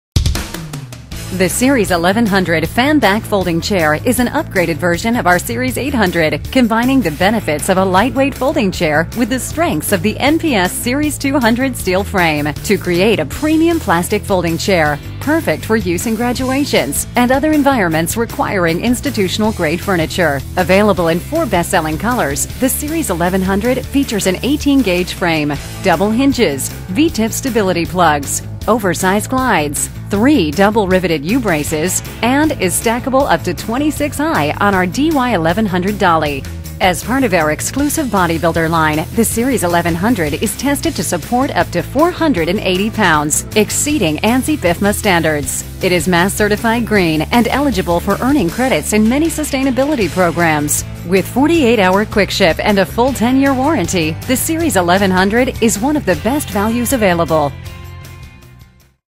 1100 Series
Why the utra comfortable NPS series 1100 chair is the perfect chair for you!